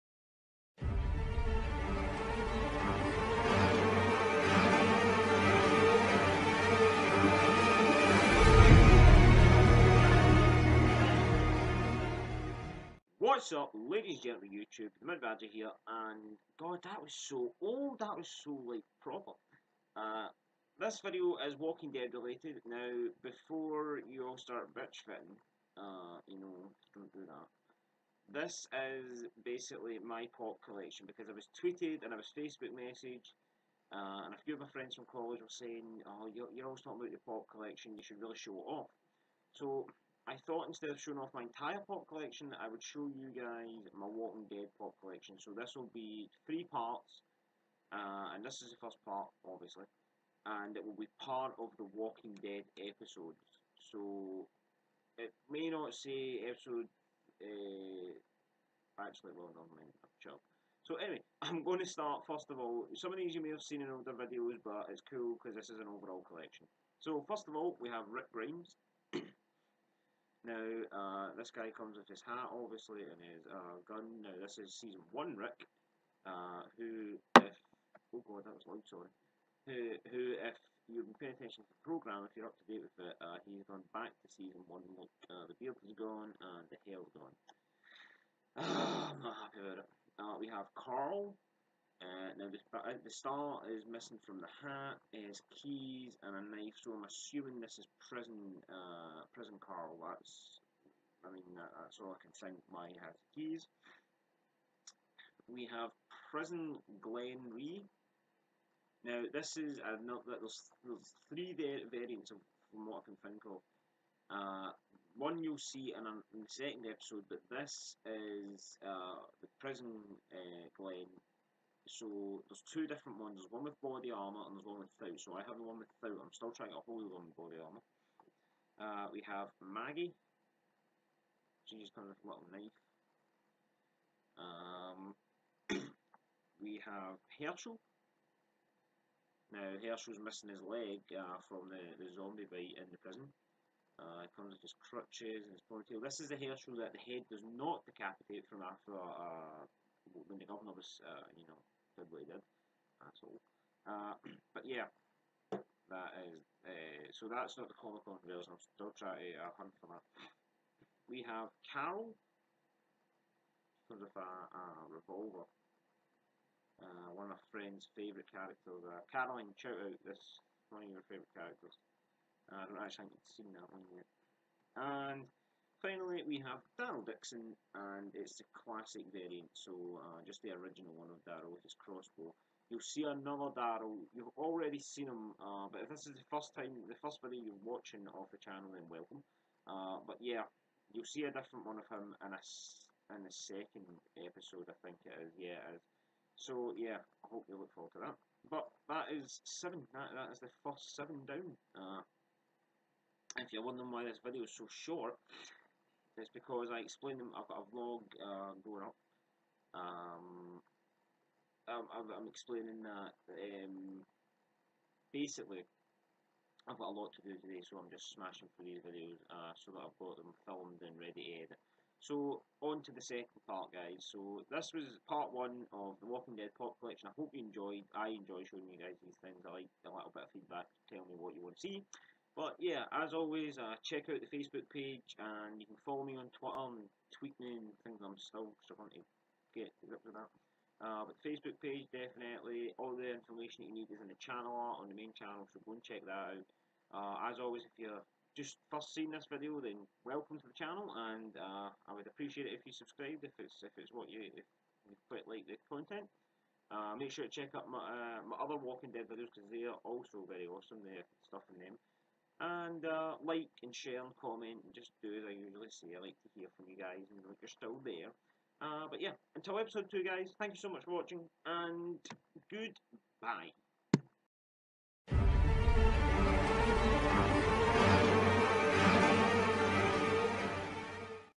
The Walking Dead pickups Ep.3
I show you guys the first 7 Funko Pop figures in my Walking Dead Collection.

Also check out the Facebook and Twitter pages.